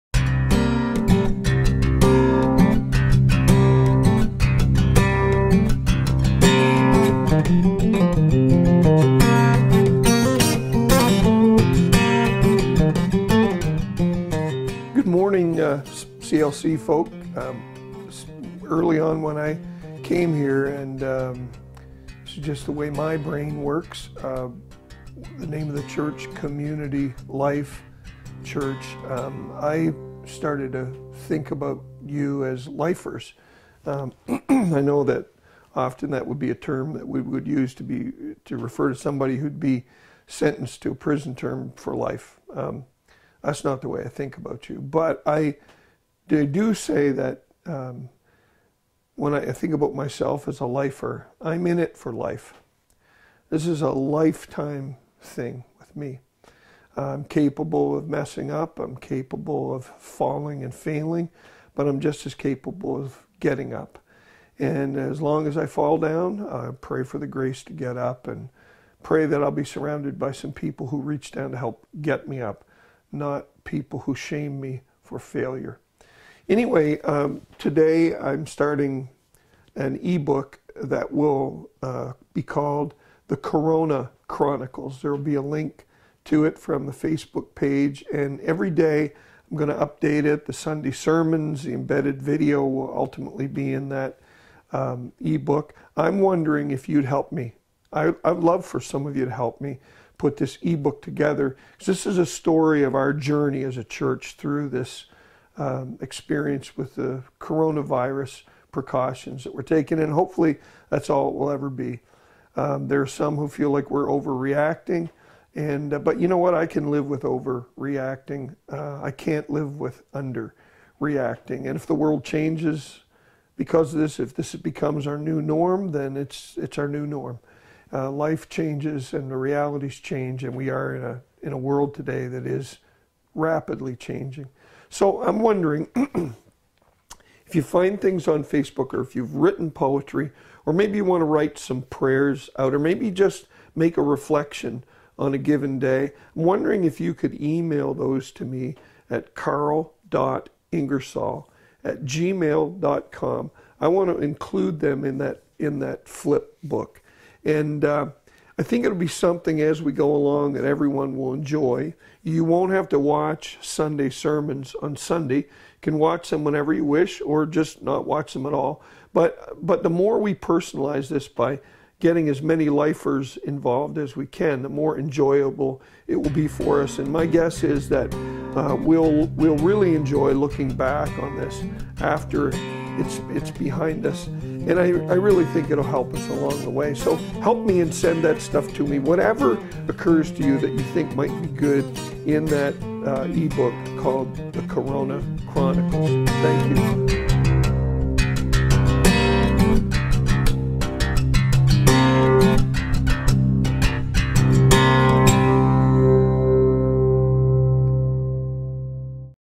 Corona Invite
An invitation to participate in an eBook Journal for Community Life Church as we journey through the coronavirus precautions